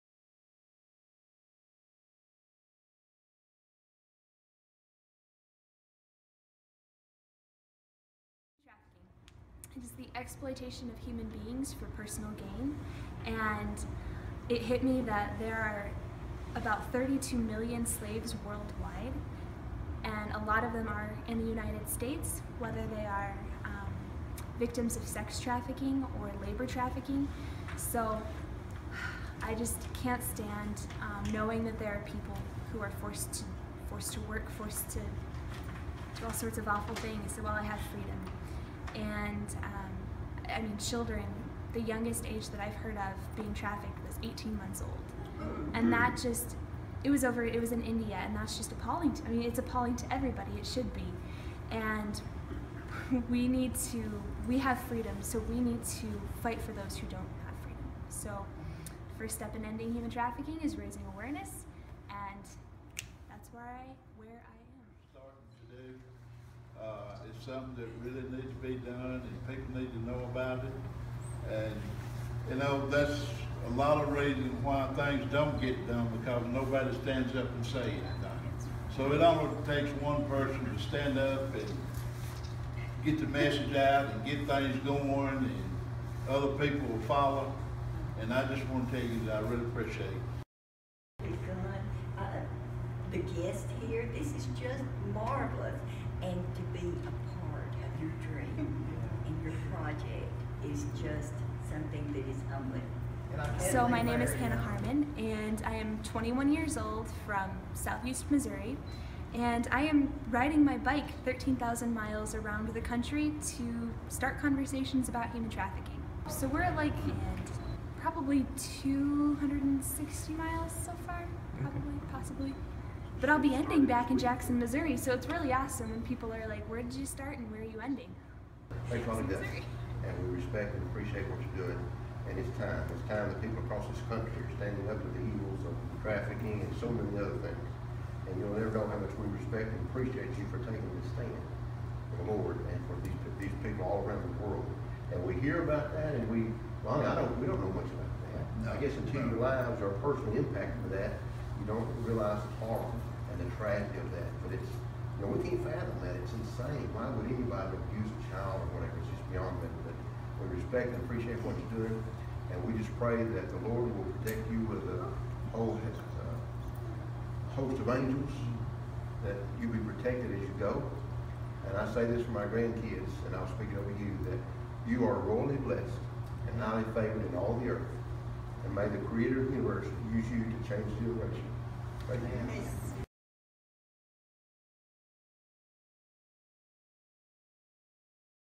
Riding against Traffic stops in Manchetser
Activist seeks to raise awareness of human trafficking by riding across America.